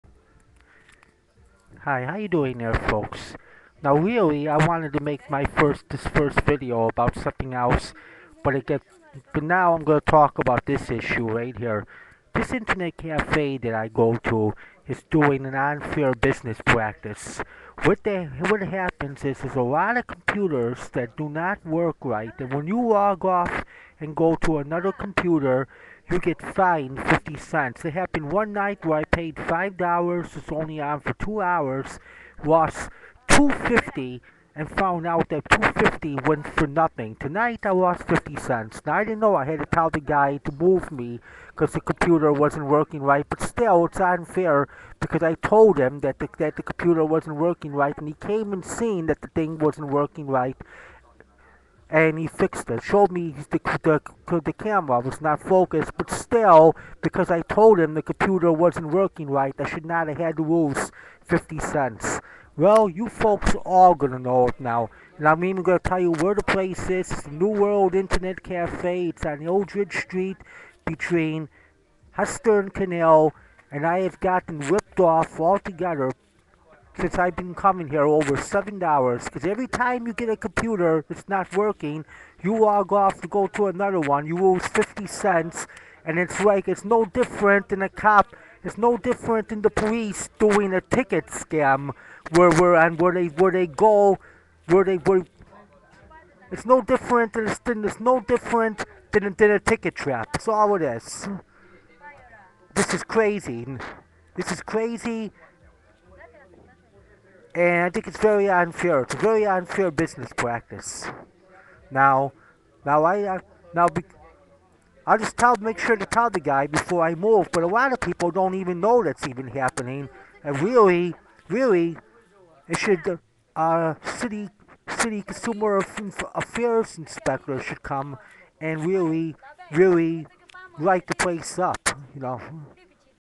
Internet cafe rips people off unknowingly
Was unfairly ripped off by new world internet cafe on Eldredge Street between Canal and Hester street on the lower East side. They are doing a unfair business practice. You log on to a computer that does not work you get fined 50 cents. This has been happening to me for months  unknowingly. kid in the backgrownd harassed me throwing spitballs. For 20 minutes owner just let it happened. Racial discrimination. I"m very close to having them thrown in jail.